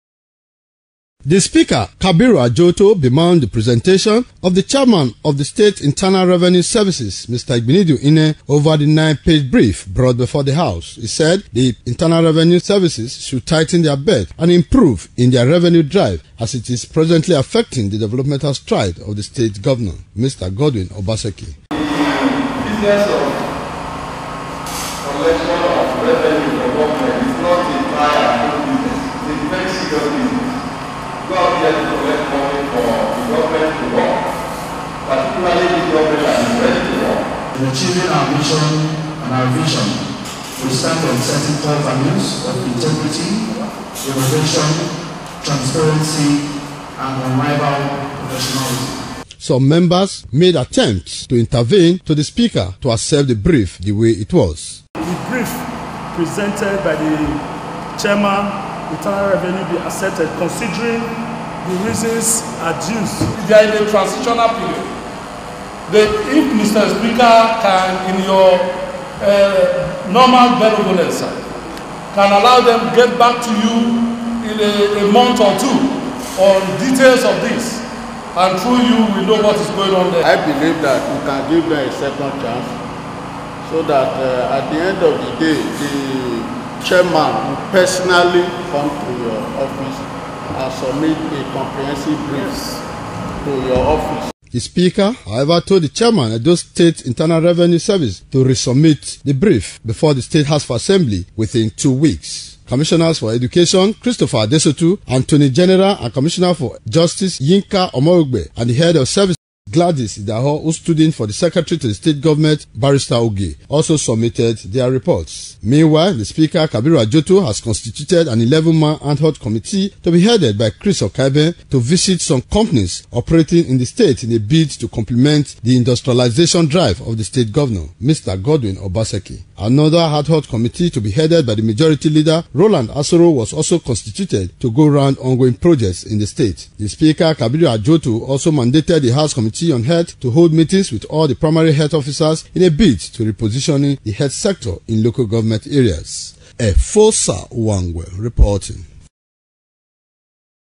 EDHA rejects brief by State Internal Revenue Services
Edo State House of Assembly on Monday rejected the brief of the State Internal Revenue Services.

Efosa Uwangue reports that the House also set up an Adhoc Committee to consolidate on the industrialization drive of the State Governor, Mr. Godwin Obaseki.

Noticing the shoddy nature of the Edo State Internal Revenue Services presentation, the speaker, Kabiru Adjoto threw the first shot at the chairman of the Services, Mr Igbinidu Inneh over the Nine Page brief he brought before the House.

However some members attempted to intervene to cover the lapses of the chairman as they try to appeal to the speaker to accept the brief the way it is even though it was not up to standard.

The appeal by the members did not in anyway allow the speaker to shift ground or compromise on the integrity and standard of the house as the chairman Edo State Internal Revenue Services brief remained rejected and to do the needful and submit within two weeks.

Meanwhile, the speaker, Kabiru Adjoto has constituted an eleven man adhoc committee  to be headed by Chris Okaiben to visit some companies operating in the state in a bid to complimenting the industrialization of the state drive of the state governor, Mr. Godwin Obaseki.

Another adhoc committee to go round ongoing projects in state to be headed by the Majority Leader, Roland Asoro was also constituted.

The Speaker, Kabiru Adjoto also mandated the House Committee on Health to hold meetings with all the primary health officers in a bid to repositioning the health sector in local government.